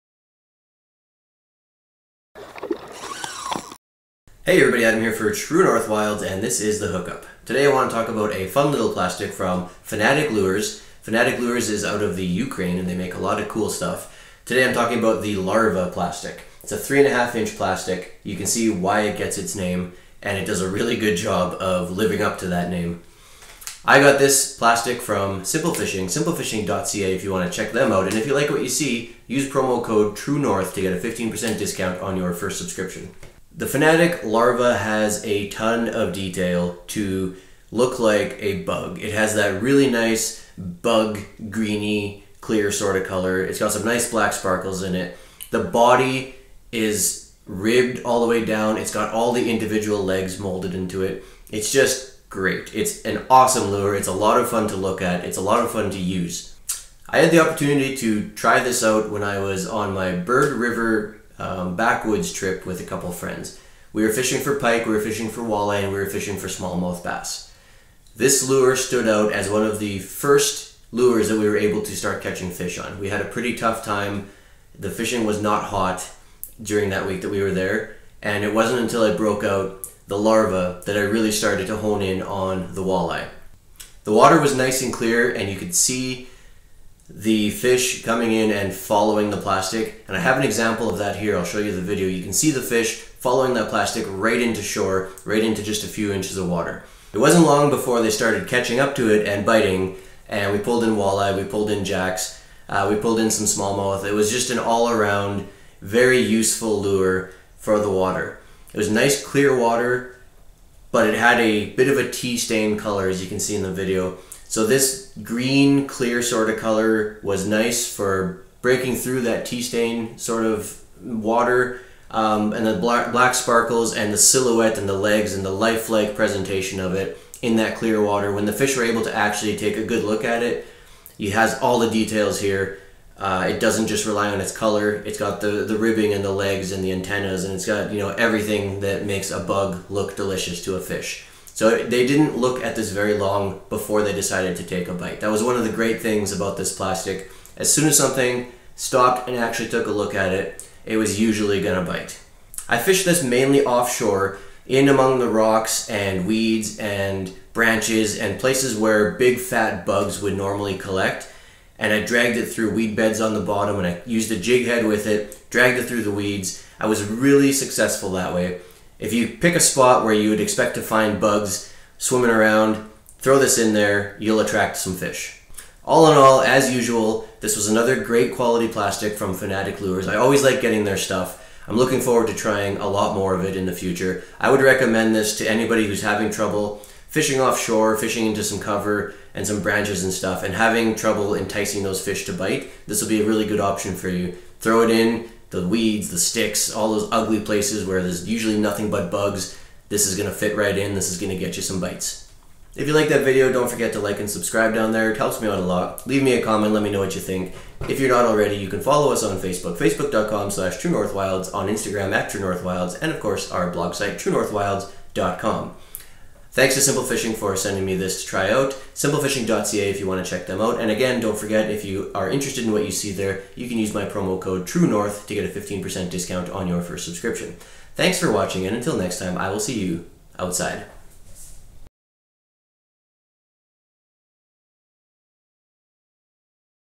Larva, fits in with the bugs, from Fanatik Baits - The Hookup
I received this lure from Simple Fishing to check them out.